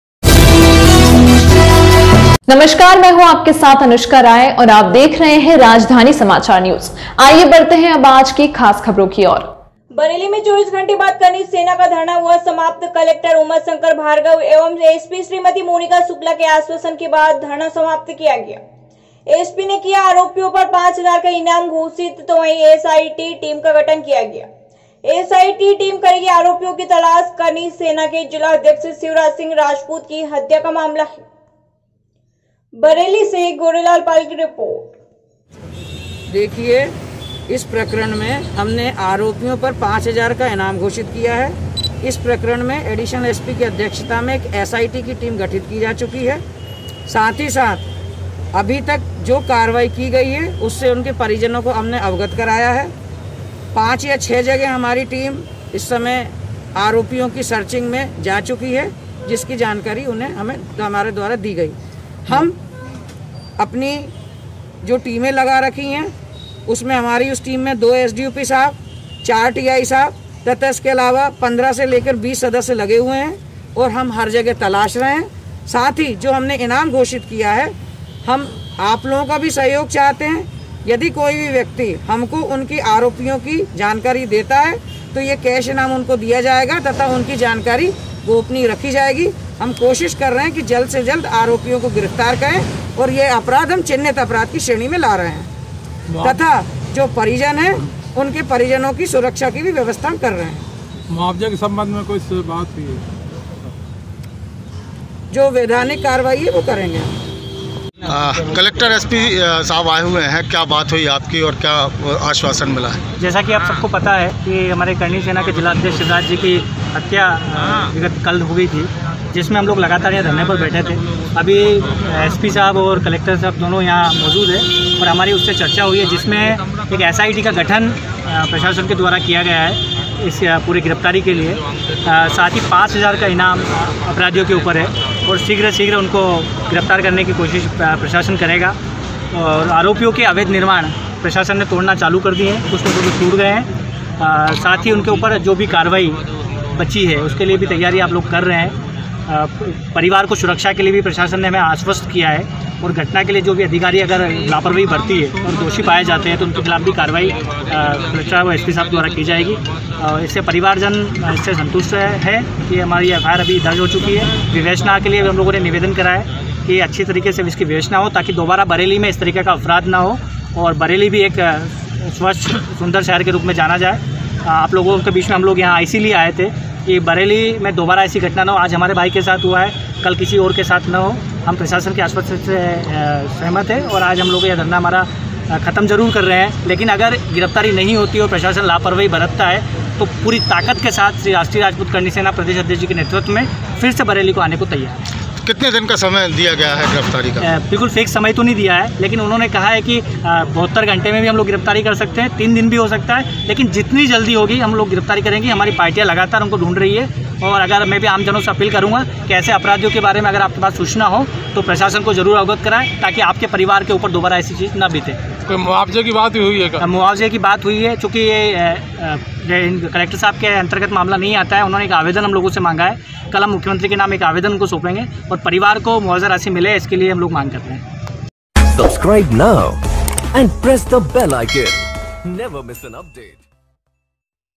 बरेली। 24 घण्टे बाद करणी सेना का धरना हुआ समाप्त आरोपियो पर पांच हजार का इनाम घोषित SIT टीम का गठन
बरेली से

बरेली में 24 घण्टे बाद करणी सेना का धरना हुआ समाप्त।कलेक्टर उमाशंकर भार्गव एवं SP श्रीमती मोनिका शुक्ला के आश्वासन के बाद धरना समाप्त।SP ने किया आरोपियो पर पांच हजार का इनाम घोषित।SIT टीम का किया गठन।SIT टीम करेगी आरोपियो की तलाश।करणी सेना के जिलाध्यक्ष शिवराज सिंह राजपूत की हत्या का है मामला।

बरेली से गोरेलाल पाल की रिपोर्ट